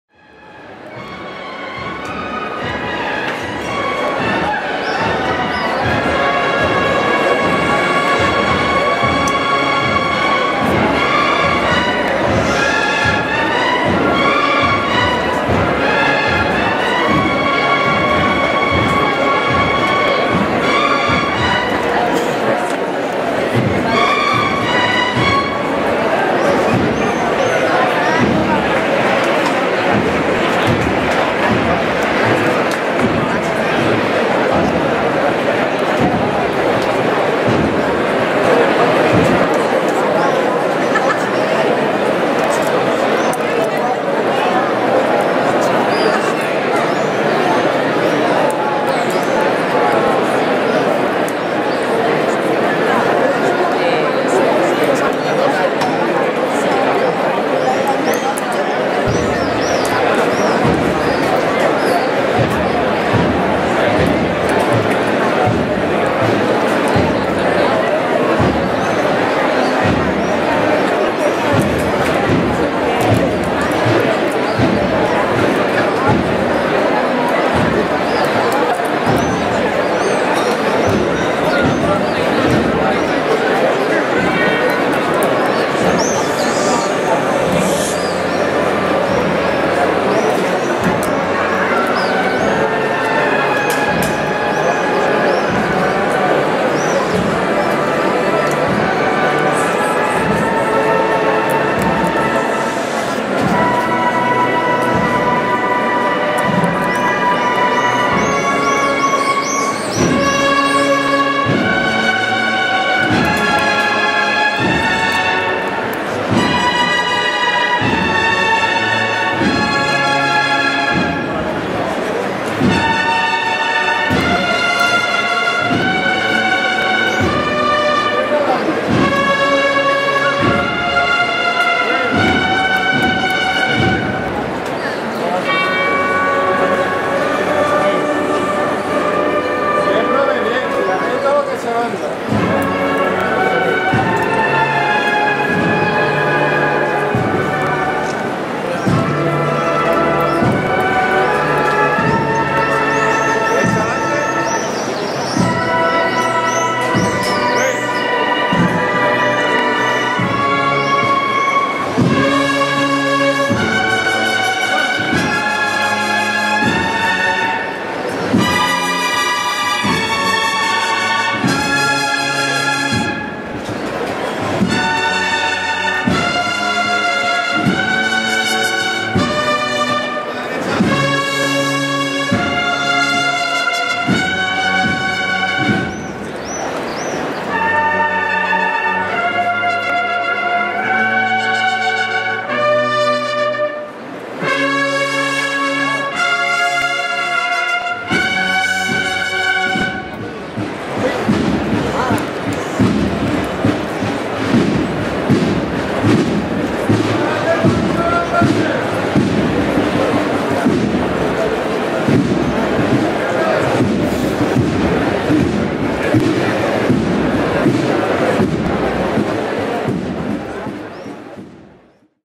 Cristo de las Aguas en la Magdalena - Semana Santa de Sevilla 2017
Suena la Banda de las Tres Caídas de Triana.